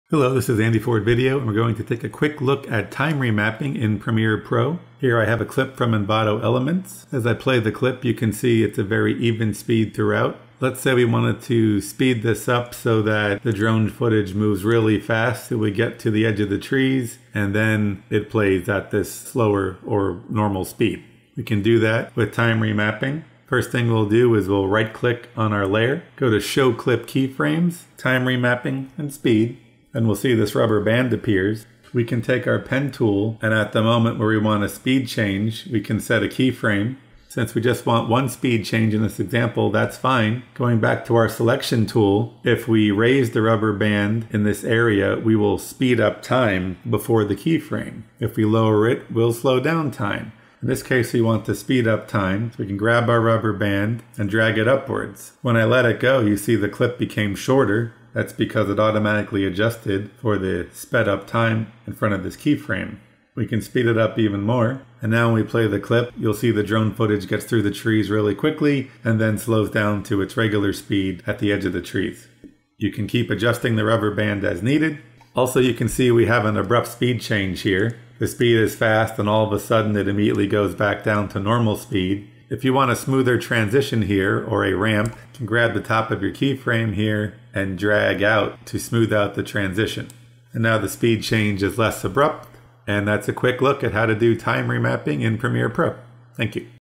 Premiere Pro Tutorial - Time Remapping
If you want to change the speed at certain spots in a singular clip, use the Time Remapping feature.

Right-click on your clip and select Show Clip Keyframes, then Time Remapping, then Speed. The rubber band becomes visible on your clip. With the pen tool, create a keyframe where you want a speed change to occur. Then, use the selection tool to pull the rubber band up (increase speed) or down (decrease speed) in the desired section of your clip.

You can add multiple keyframes and change the speed in numerous segments of your clip. To provide a smoother transition between speed changes, you can click and drag the top of a keyframe to create a ramp. This makes the speed change less abrupt.